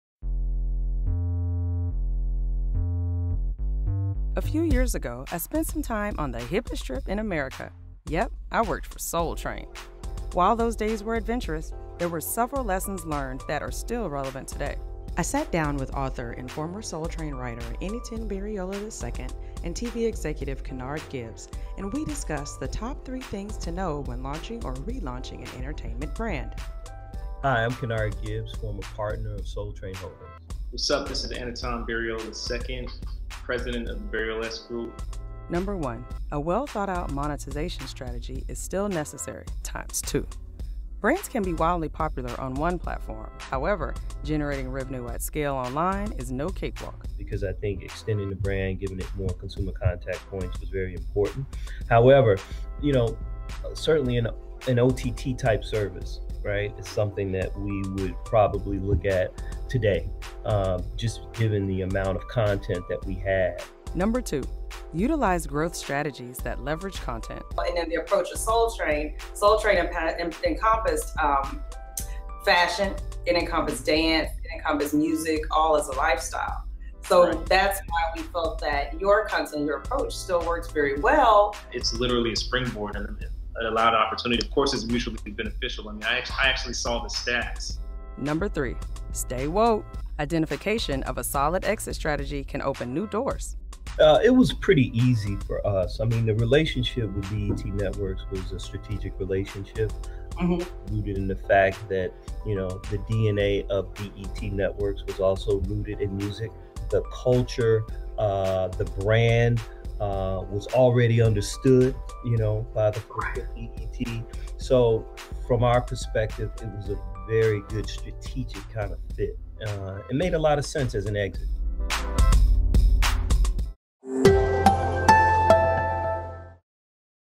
Ask the Expert: “Launching a Brand: 3 Things to Know” | AT&T
Idyllic Interactive founder and start-up marketing expert Arisha Smith talks to television executive Kenard Gibbs and content writer Enitan Bereola II about three things to consider when launching or re-launching a brand.

Find more expert tips on doing it big on digital